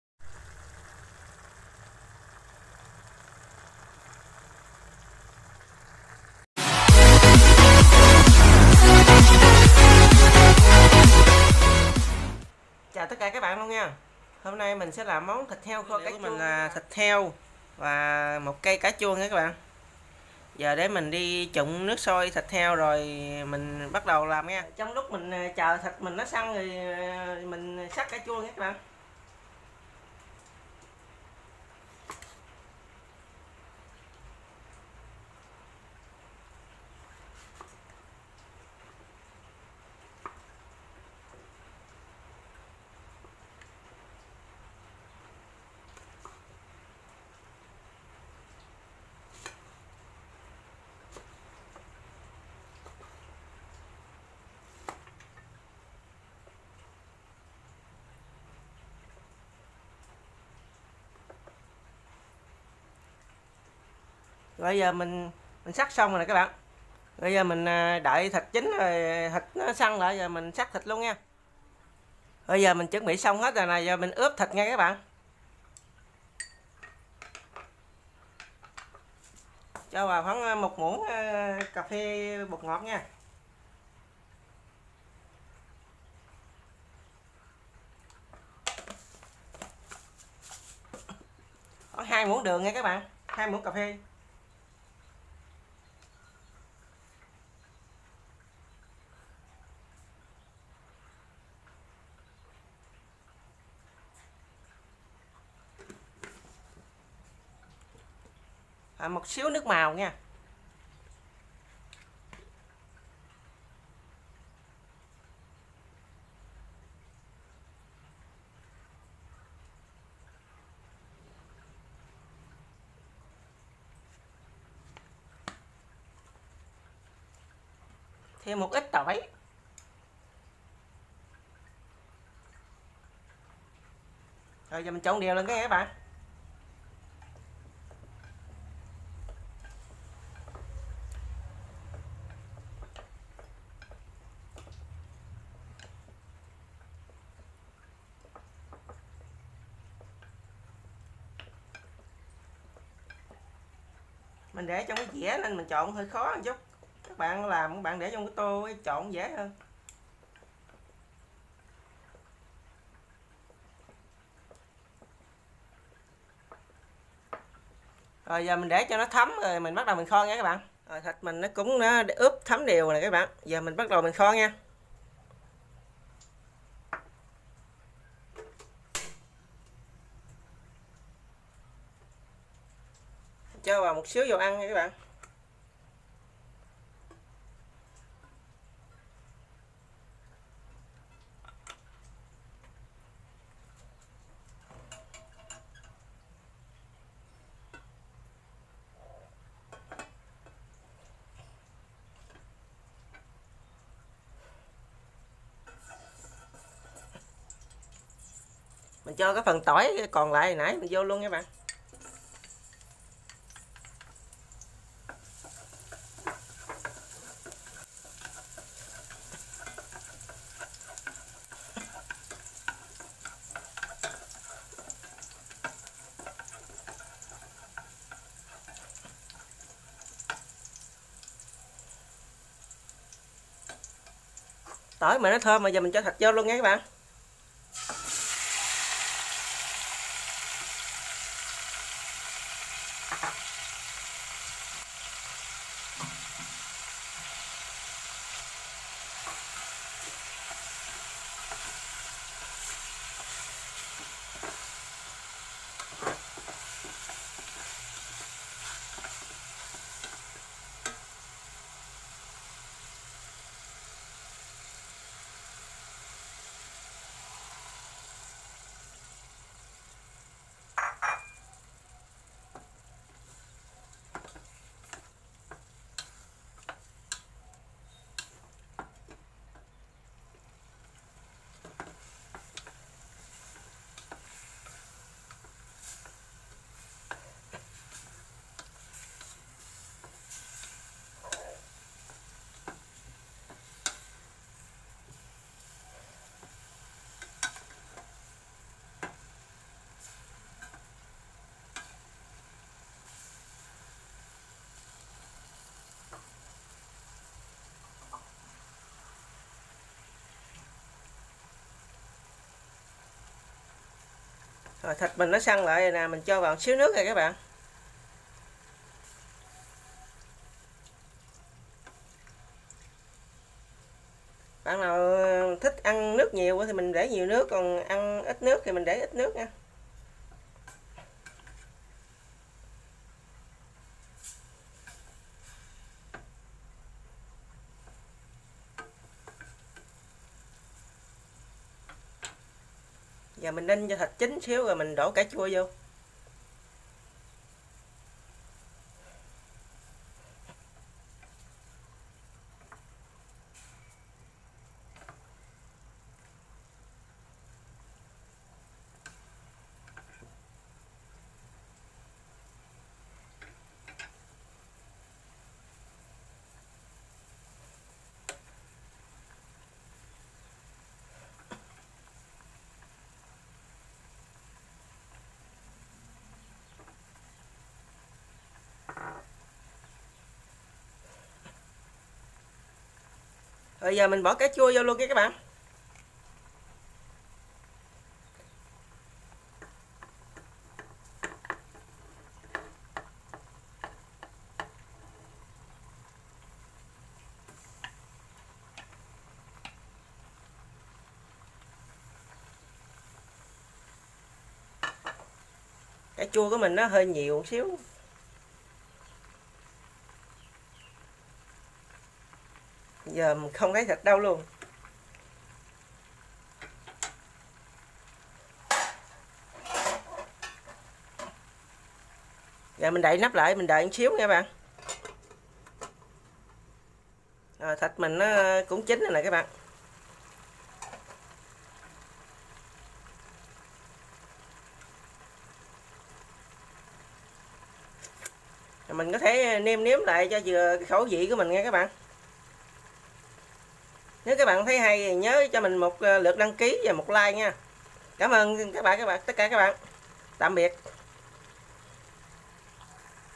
thịt heo kho cải chua - hướng dẫn làm món heo kho cải chua món ngon miền tây | lú miền tây
nguyên liệu của mình gồm có
_ 500g thịt ba chỉ hay ba rọi
_ 1 cây cải chua
_ các gia vị đi kèm như muố đường bột ngọt.
sơ chế
_ thịt heo rửa sạch luộc sơ qua với nước sôi sau đó cắt miếng vừa ăn sau đó uớp gia vị để khoản 15 phút
_ cải chua cắt nhỏ
chế biến
_ cho chảo lên bếp phi tỏi và hành tím vàng sau đó cho phần thịt đã ướp vào sào săn lại cho vào xíu nước ninh khi thịt gần chín thì cho phần cải chua vào nấu thêm 10 phút nửa .hết
chúc cac bạn thành công nếu thấy hay xin cho mình 1like và 1 lượt đăng ký kếnh nhé